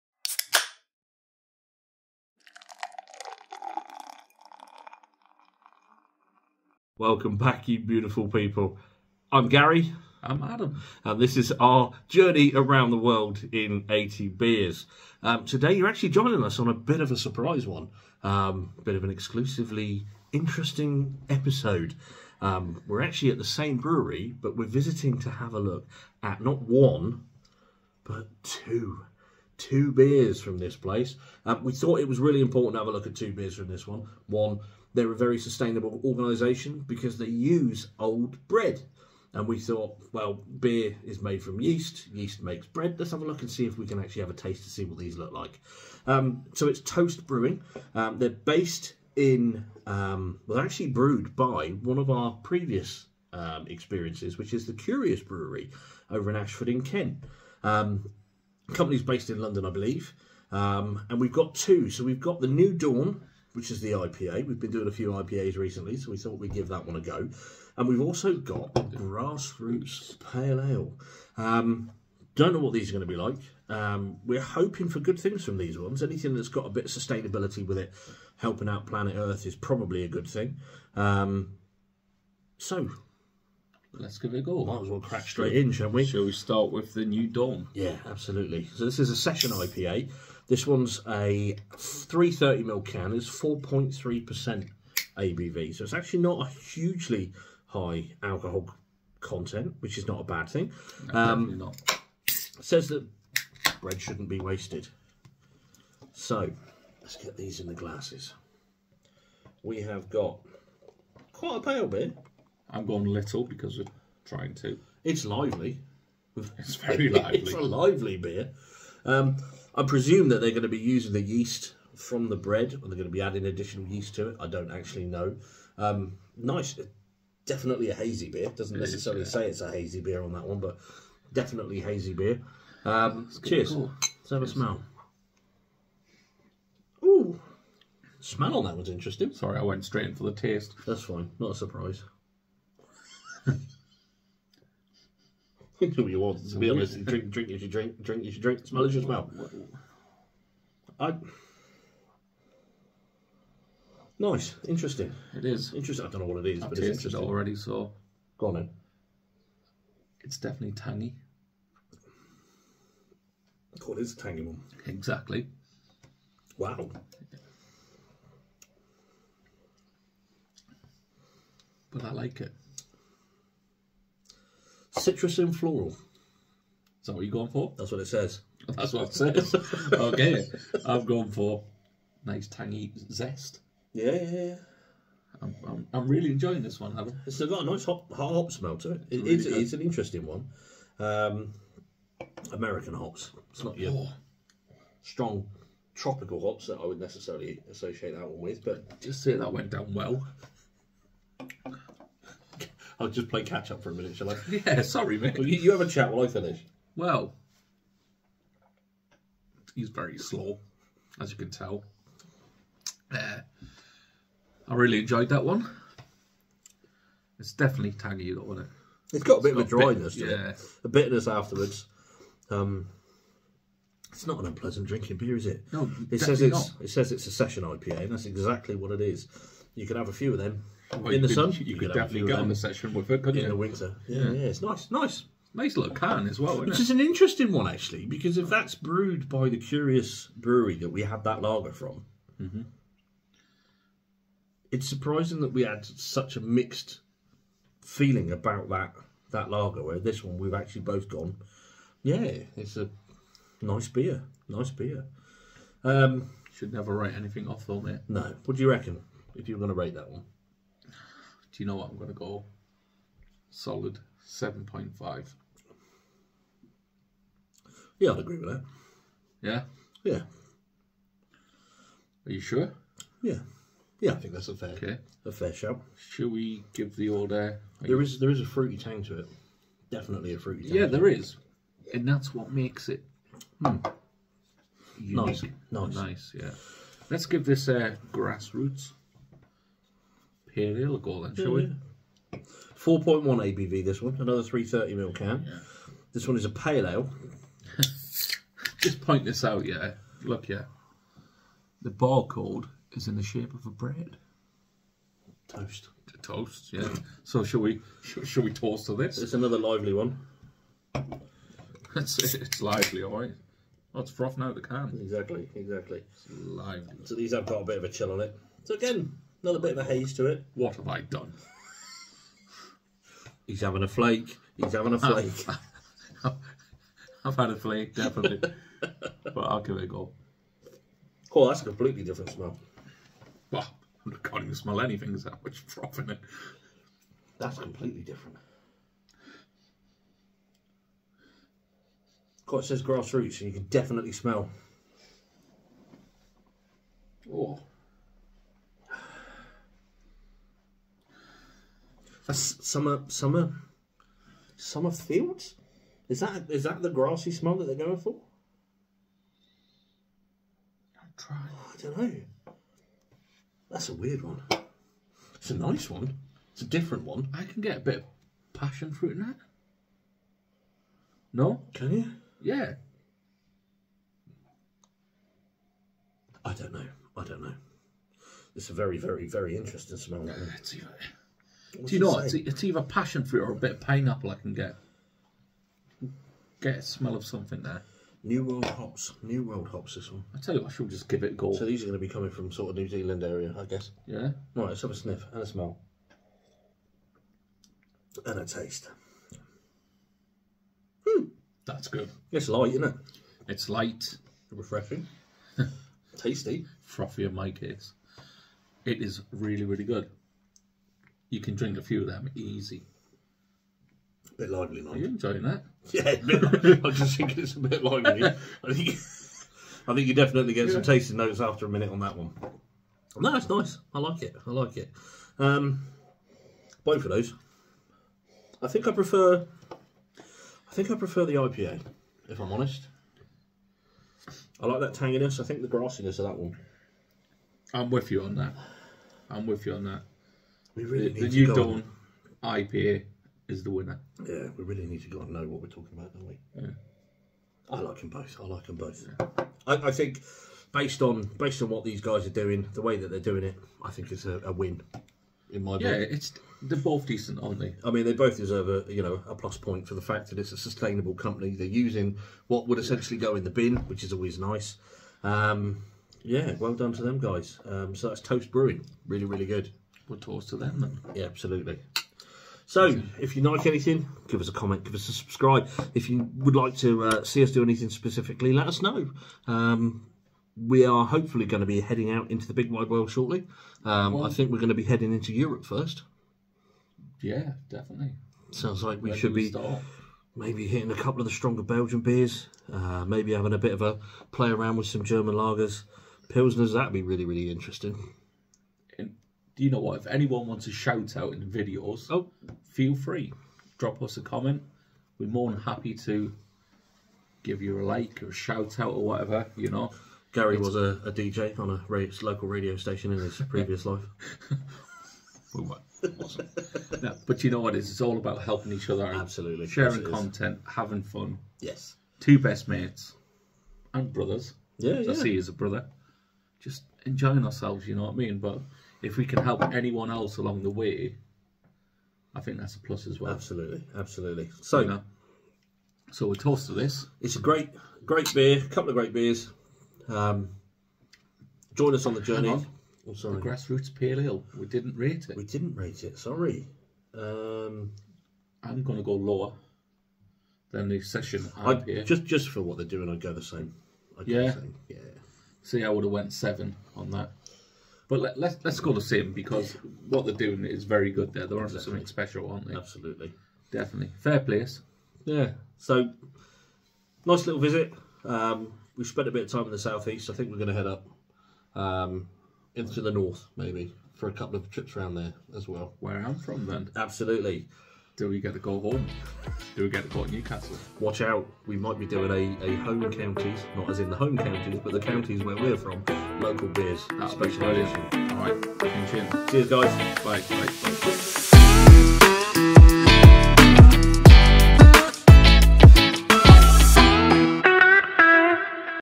Toast Brewing New Dawn IPA and Grass Roots Ale - Beer Review No. 6 - Around the World in 80 Beers
This week we try two beers from Toast Brewing. We compare these two sustainably brewed beers on the next stage try 80 beers from around the world.

Please see our other socials for more updates and extras.